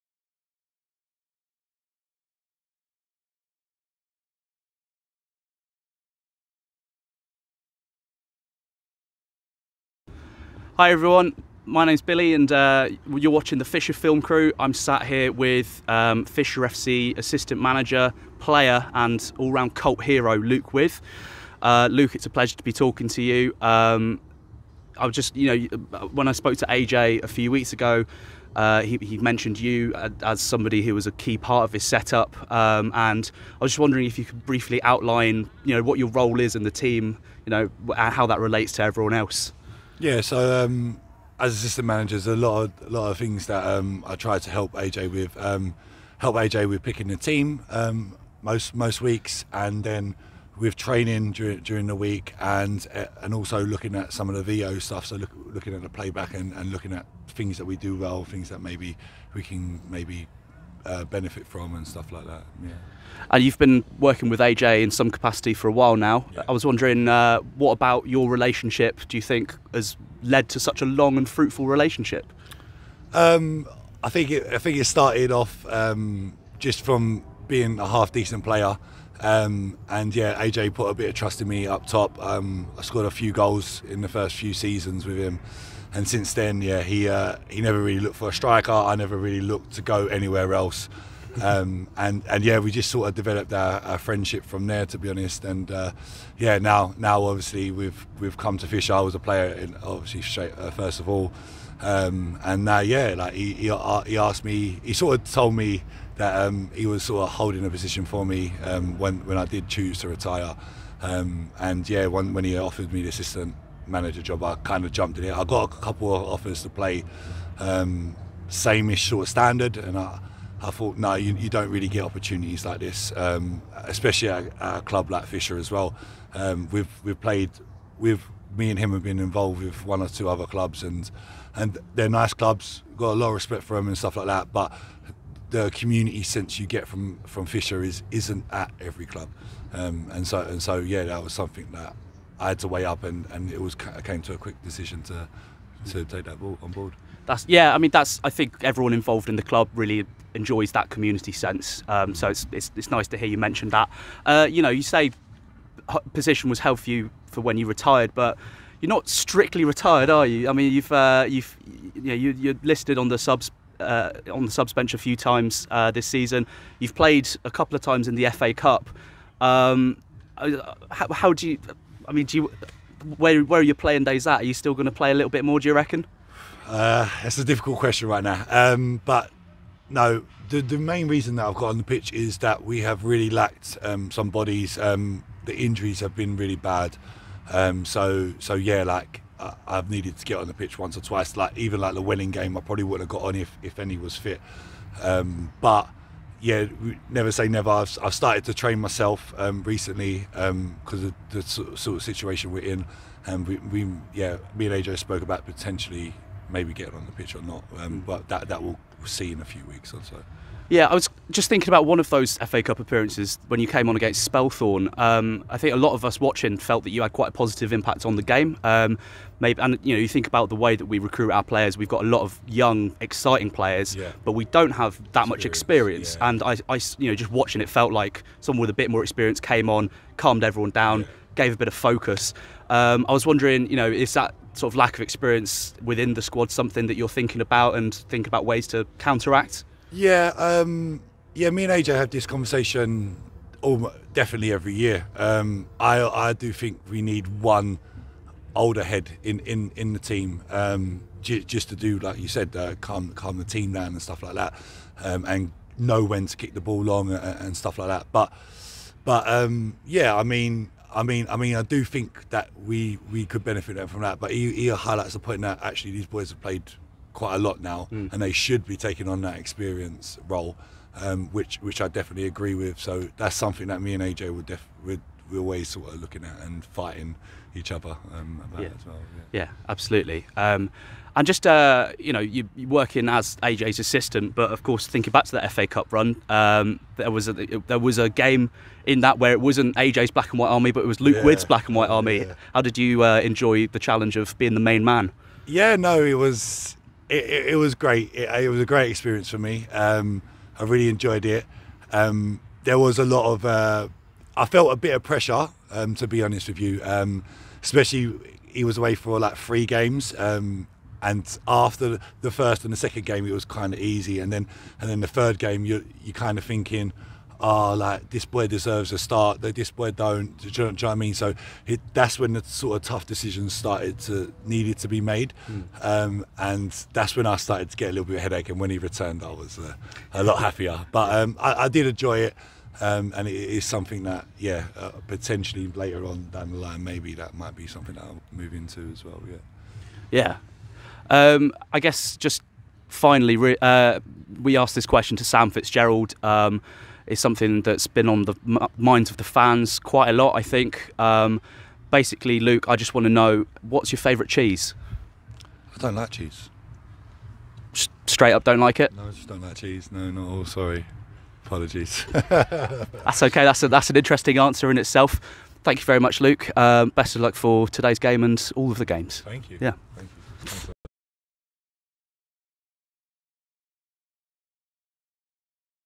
When Billy met Luke
Fisher Film Crew's Billy sits down with Fisher FC assistant manager Luke With